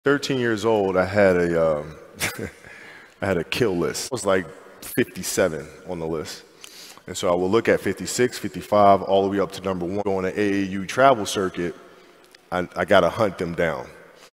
Kobe Bryant 🏀 Kill-List at 13 years old. Black Mamba Mindset.
At 13 years old, Kobe Bryant was 57th hottest young prospect in the nation. He made sure to face everyone above him on the list one by one. Different class. Black Mamba mentality.